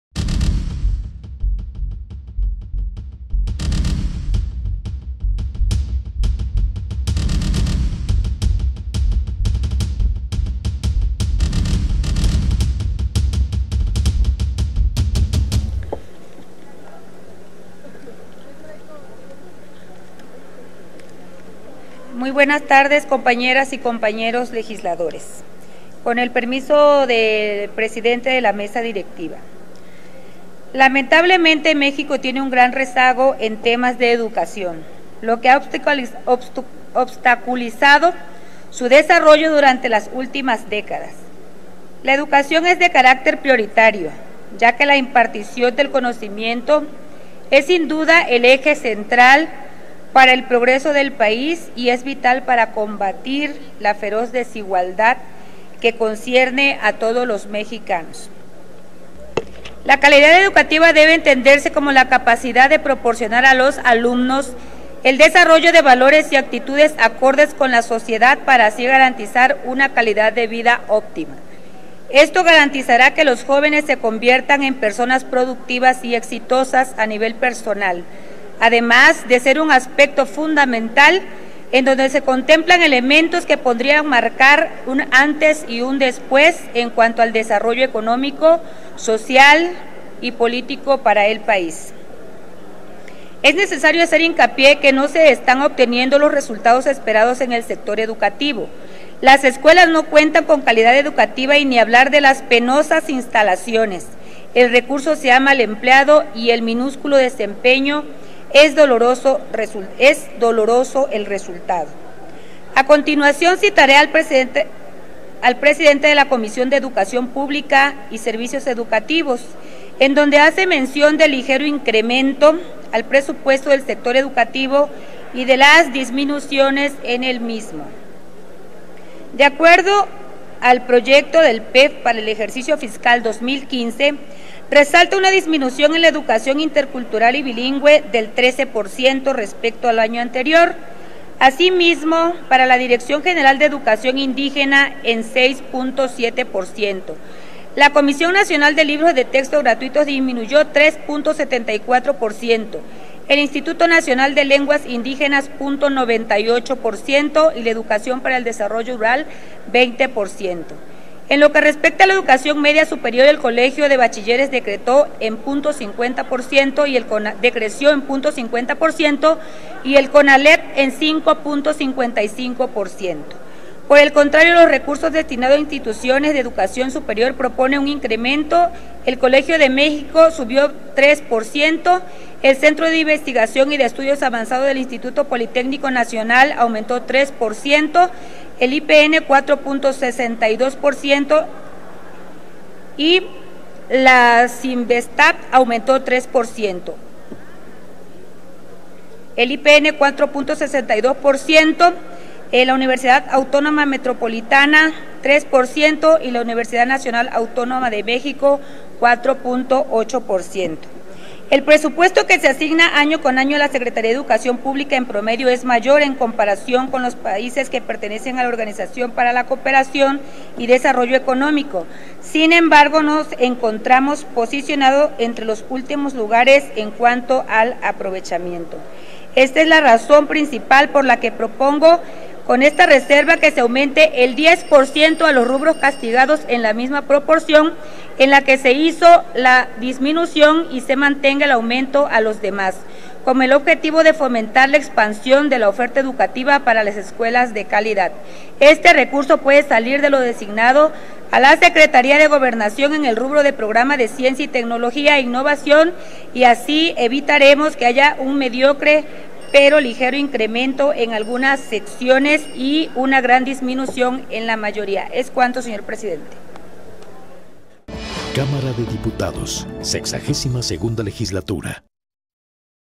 Dip. Lorena Méndez (MC) - Presupuesto de Egresos de la Federación 2015 (Reservas)
La Dip. Lorena Méndez Denis (MC) destaca la importancia de la educación y a pesar de ello no se ha invertido en las escuelas que tienen carencias. Cita al presidente de la Comisión de Educación y Servicios Educativos, quien mencionó los mínimos aumentos que hay en el rubo, por lo que propone aumentos para fomentar la expansión de la oferta educativa de calidad.